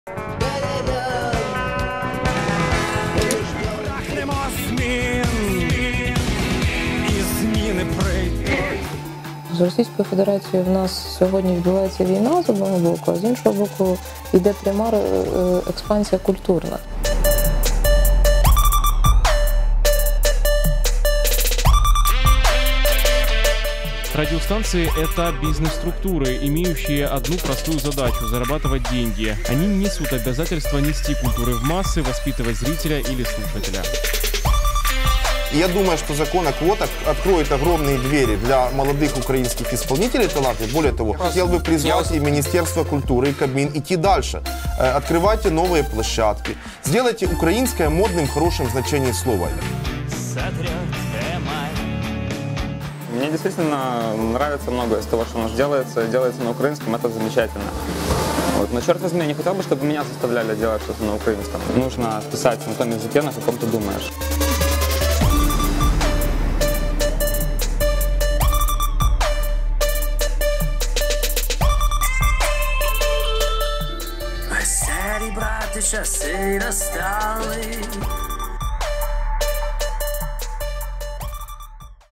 Сколько должны петь и говорить на украинском в национальном эфире?
С прошлой недели украинское радио – в жестких языковых рамках. В силу вступил закон, который указывает, сколько должны петь и говорить на украинском в национальном эфире. И проверяющие намерены строго следить за тем, как он исполняется.

Родного языка на украинском радио слишком мало, решили власти. В конце июля президент Украины подписал изменения в Закон "О телевидении и радиовещании", в котором было сказано, что каждая третья песня в эфире украинских радиостанций отныне должна быть на украинском языке. Для ведущих языковые квоты сделали еще более строгими: они отныне должны говорить на национальном языке половину своего эфирного времени.

Изменения в Законе "О телевидении и радиовещании" вступили в силу в начале ноября. И украинский радиоэфир с начала месяца находится под строгим языковым контролем. В национальном совете по вопросам телевидения и радиовещания пишут все радиопрограммы, для чего установили 32 магнитофона – для каждой радиостанции свой.

Часть украинских артистов согласна с введением квот и считает, что песен на украинском в эфире было недостаточно. Они также жалуются, что раньше в эфире пробиться было практически невозможно из-за того, что радиостанции предпочитали зарубежную попсу.

Первые результаты проверки радиостанций на языковые квоты Нацсовет представит через неделю. За несоблюдение закона о квотах станции будут штрафовать.